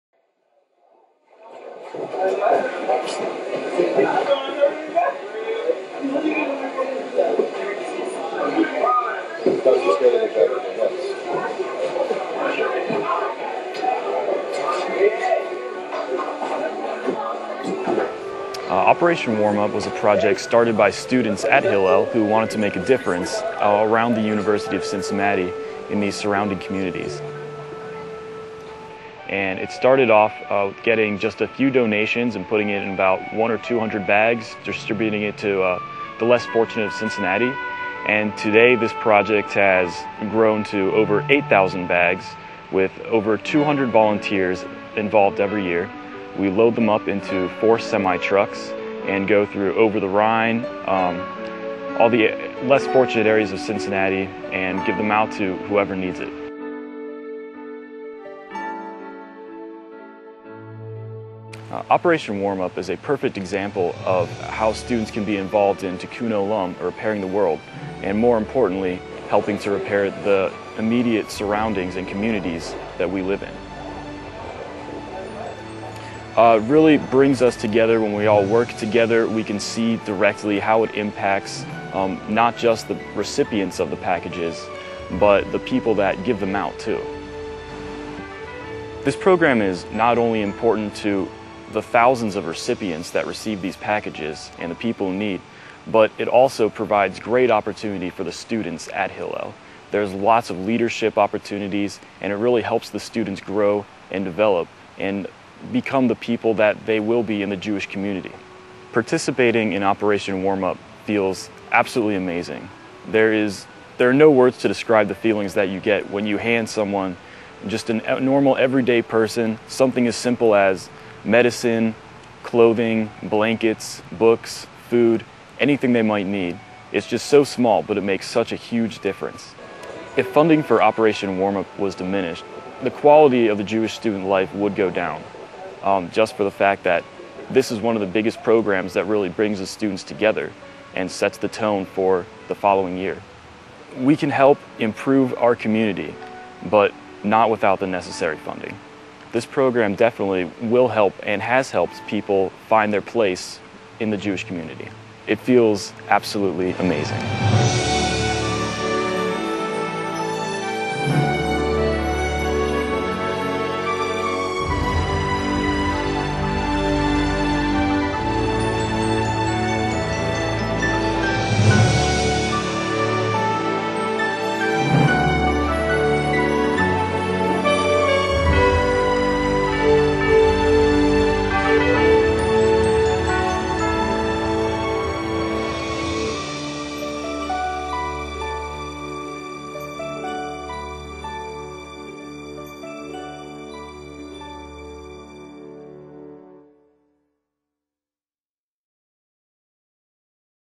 Meet Nate and Be Inspired
Nate Dumtschin, a student from the University of Cincinnati, wanted to connect to the Jewish Community and make a difference. He took a leadership role in the Hillel Jewish Student Center's "Operation Warm-Up."

"There are no words to describe the feelings that you get when you help someone, just a normal everyday person, in something as simple as medicine, clothing, blankets, books, or food."

Today, Operation Warm-Up has grown from distributing 200 care packages to over 8,000, and engages over 200 volunteers every year.

"This program is not only important to the thousands of recipients that receive the packages. It is a perfect example of how the students can be involved in Tikkun Olam, and, more importantly, how we can repair the immediate surroundings and communities that we live in."

The majority of Jewish young adults indicate that making the world a better place is a very important aspect of their Jewish identity.

- 78% of Greater Cincinnati Jewish young adults want to increase their Jewish Connections.*

- In six years, 168 Mitzvah Challenge participants (B'nai Mitzvah age) donated $121,540 to organizations in Cincinnati, Israel and around the world.

- About 400 Jewish young adults have participated in social action projects through the Young Adult Division of the Jewish Federation of Cincinnati (YAD).

* The 2008 Cincinnati Jewish Community Study

The Jewish Federation of Cincinnati supports social action projects that make our community a better place to live.